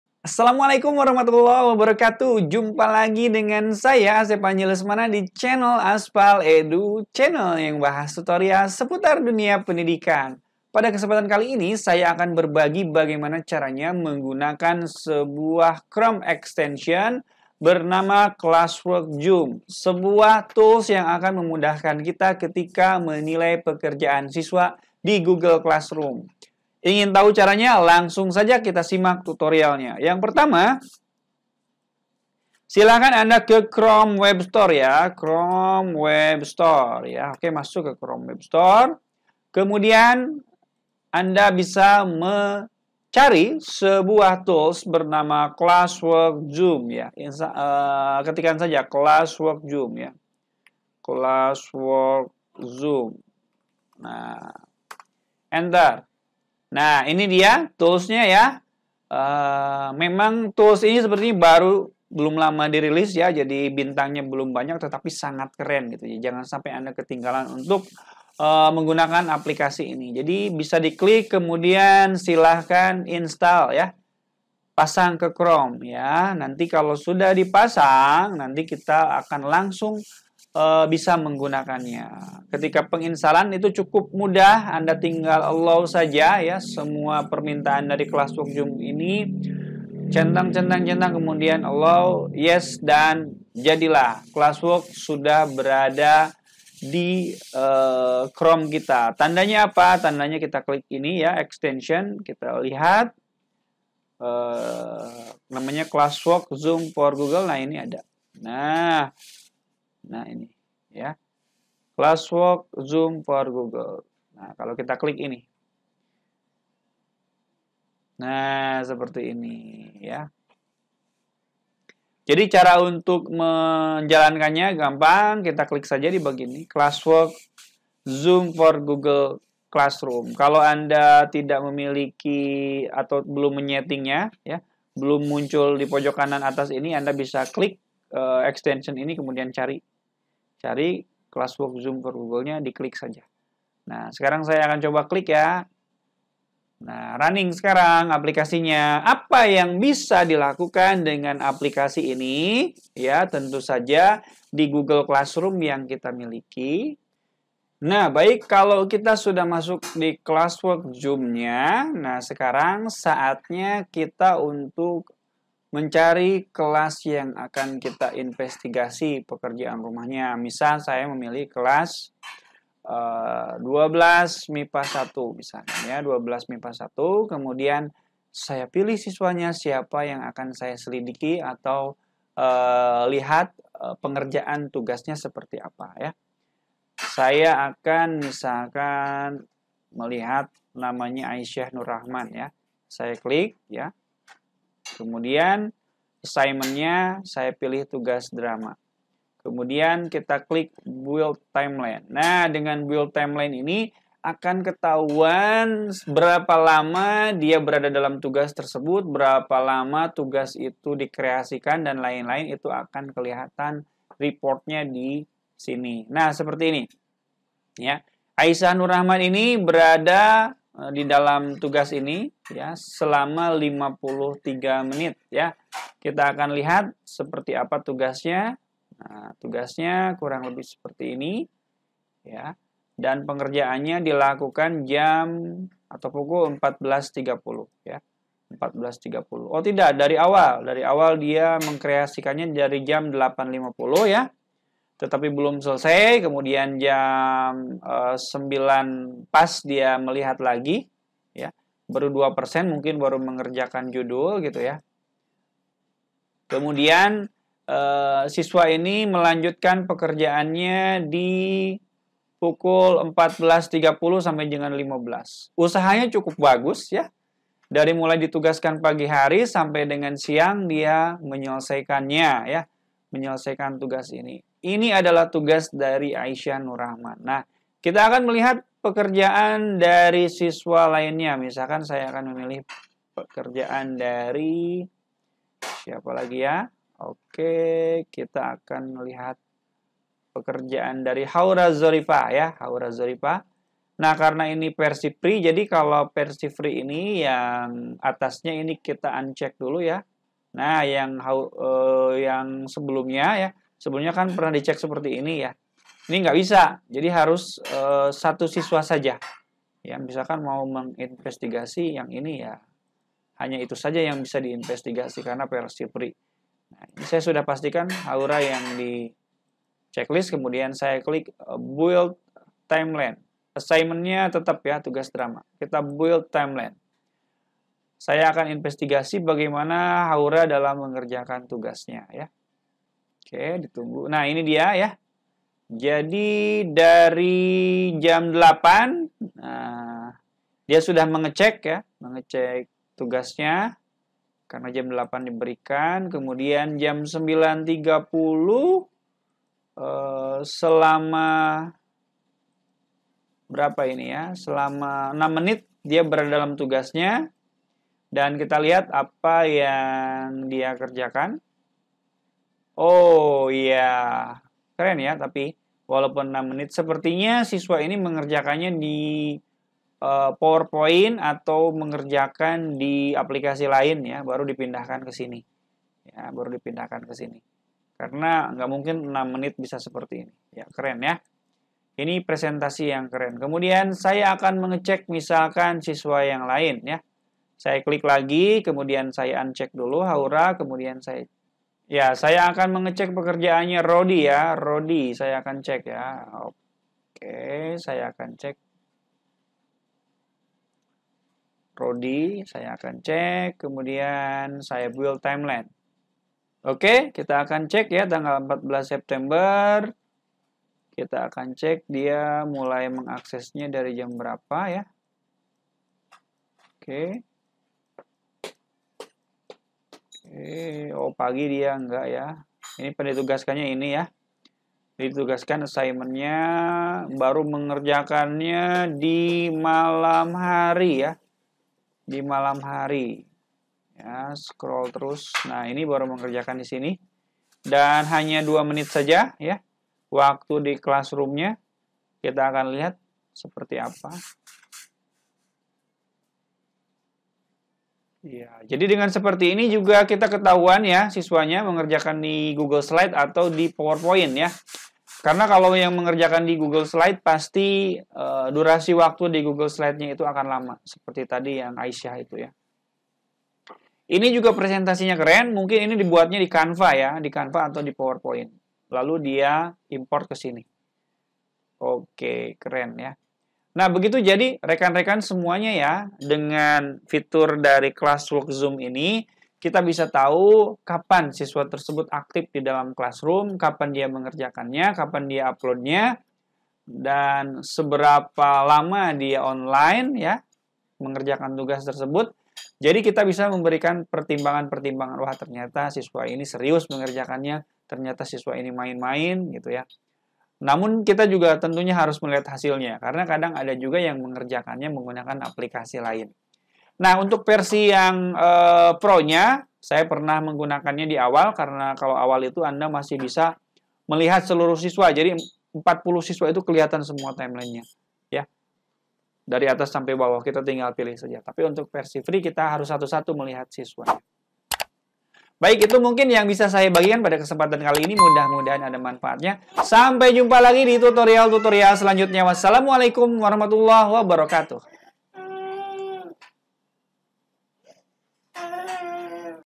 CARA CEK HISTORI TUGAS SISWA DENGAN CLASSWORK ZOOM
Pengguna google classroom memang bisa melihat hasil pekerjaan siswa, namun bagimana histori pengerjaan tugas itu, kapan siswa mulai, dan berapa lama waktu yang dibutuhkan sampai saat ini belum ada di fitur google classroom. Nah dengan bantuan classwork zoom kita bisa melakukan semuanya. Berikut ini tutorialnya.

Pada video ini juga dibahas CARA CEK HISTORI TUGAS SISWA DENGAN CLASSWORK ZOOM, tutorial classwork zoom, dan cara cek durasi pengerjaan tugas di classroom